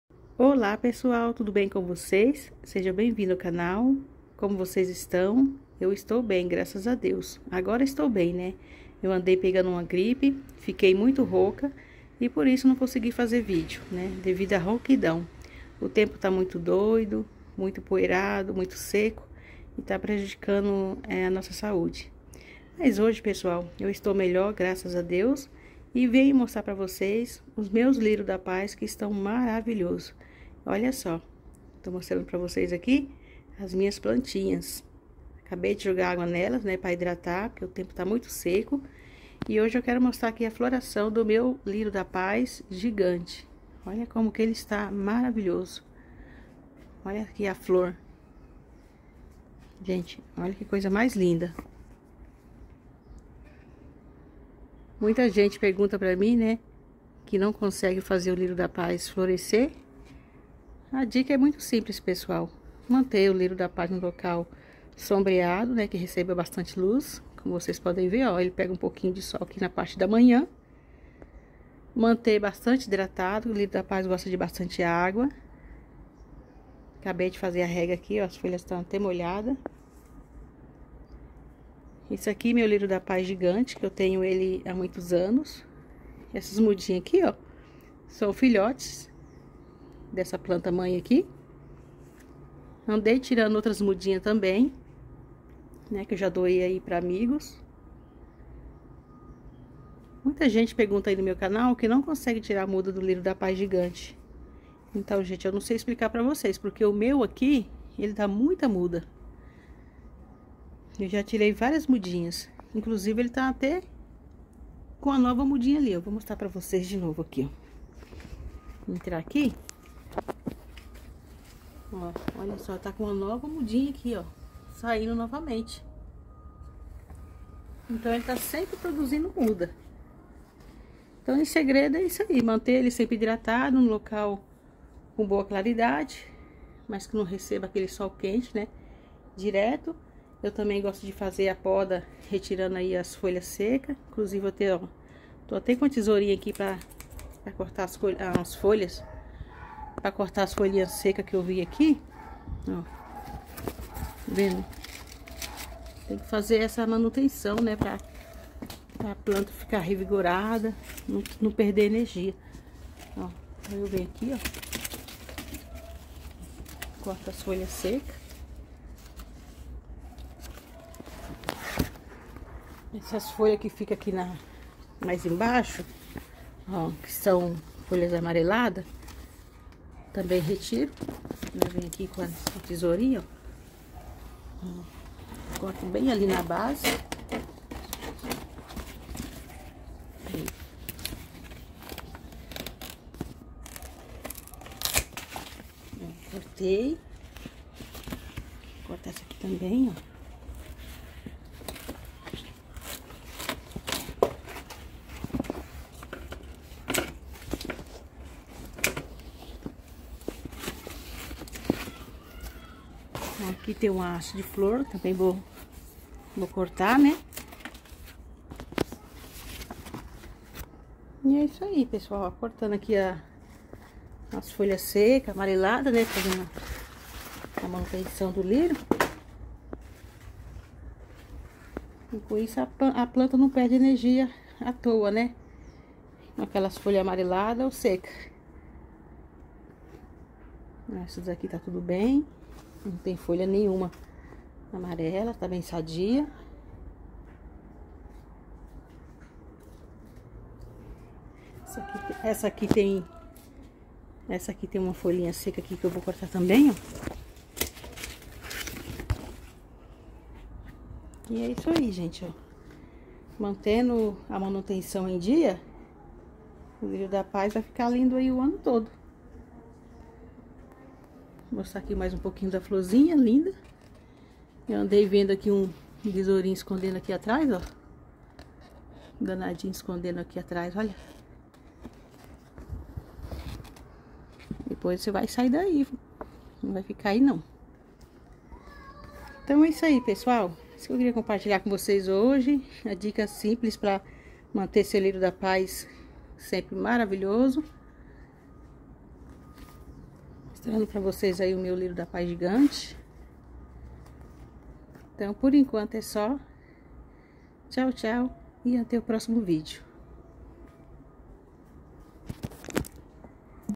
👉 Como deixar LÍRIO DA PAZ Saudável e FLORIDO Sem Complicação!!! 🌸🌿
Neste video estou mostrando os lírios da paz gigante do meu quintal. também passo dicas de como cuidar para ter lirios da paz sempre lindo e maravilhoso.
Muito obrigado, volte sempre!! :)